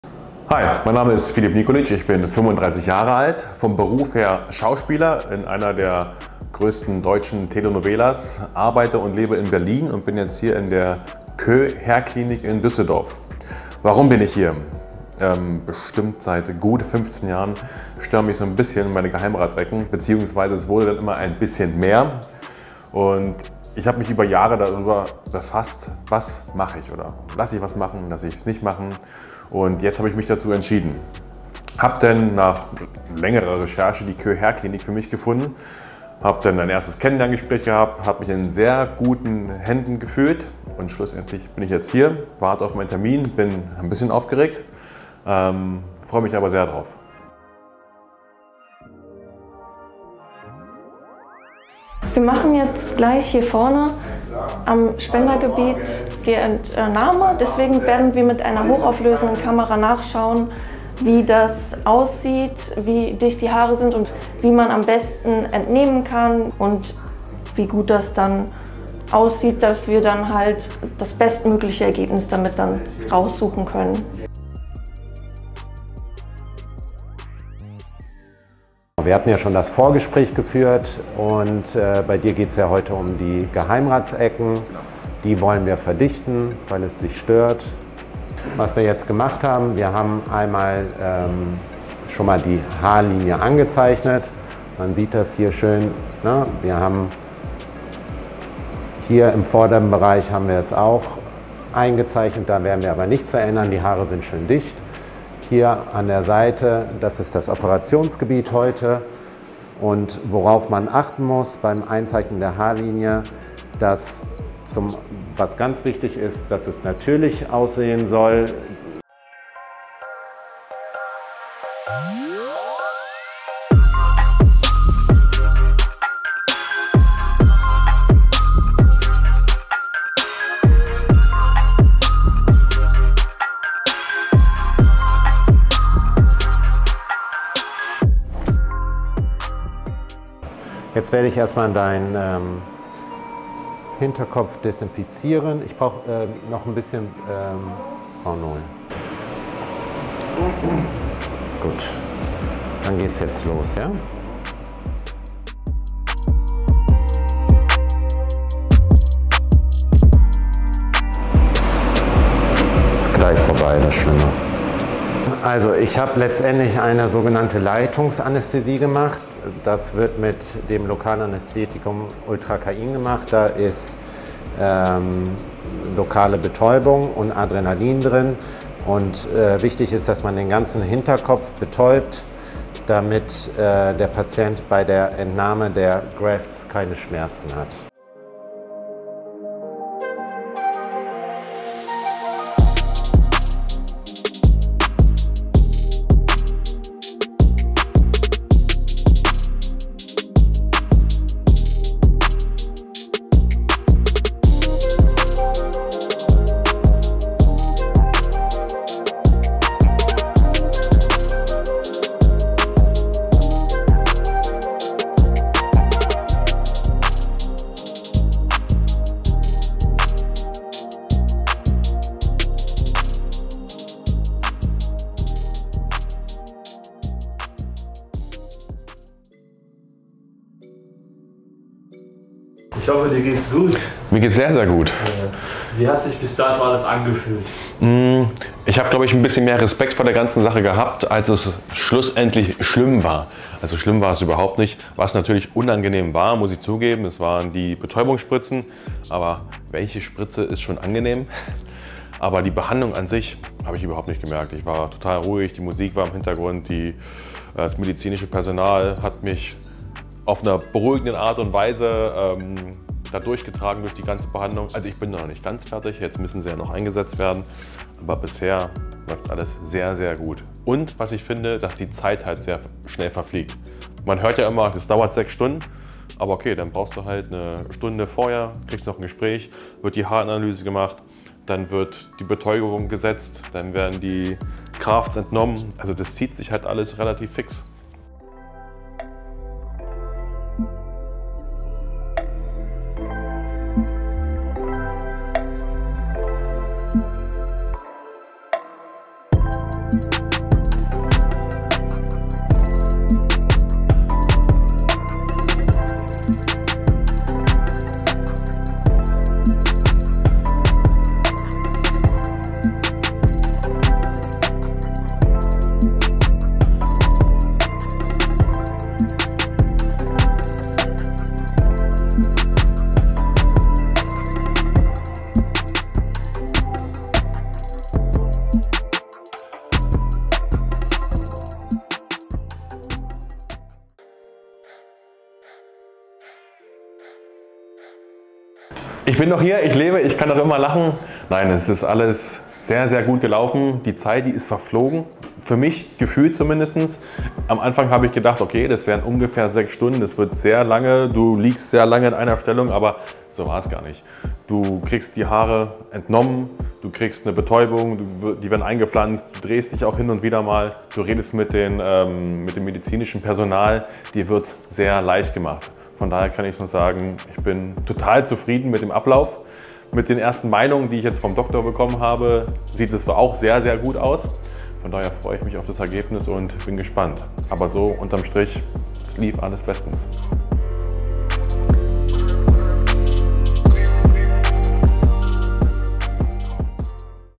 Haartransplantation bei KÖ-HAIR: Filip Nikolic unterzieht sich einer Haarverpflanzung
Seinen Traum von einer Haartransplantation erfüllt sich Schauspieler Filip Nikolic bei KÖ-HAIR in Düsseldorf. Wir haben den Berliner bei seiner Behandlung begleitet.

Bei Fragen stehen wir Ihnen unter der Telefonnummer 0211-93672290 an Werktagen von 08:00 Uhr bis 18:00 Uhr zur Verfügung.